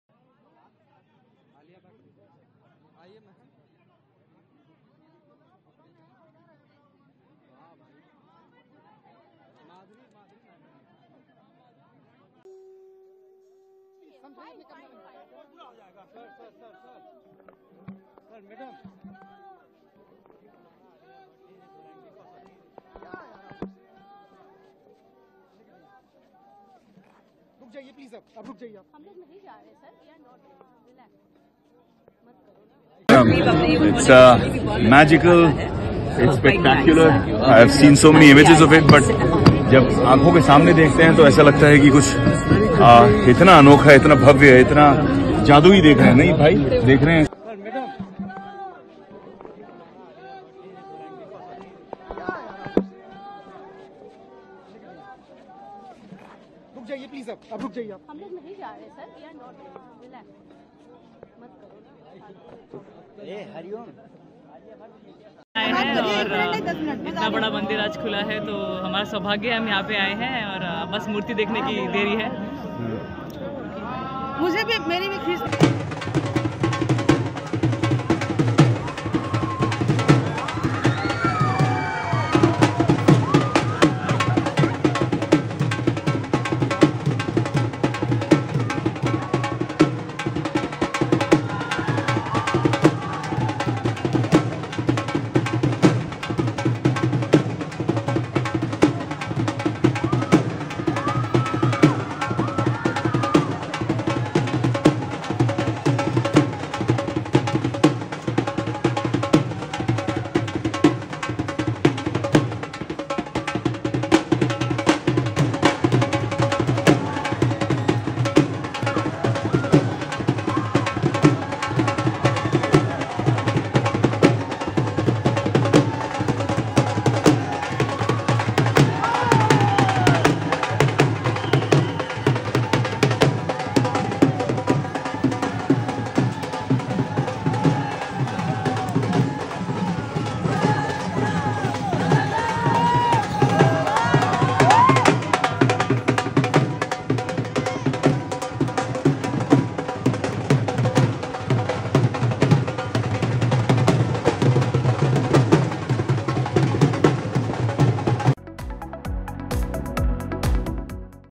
Ram Mandir Live:: Alia Bhatt-Ranbir Kapoor, Vicky Kaushal-Katrina समेत सितारें पहुंचे मंदिर
Ram Mandir Inauguration Live: Alia Bhatt-Ranbir Kapoor, Vicky Kaushal-Katrina समेत सितारें पहुंचे मंदिर | News Tak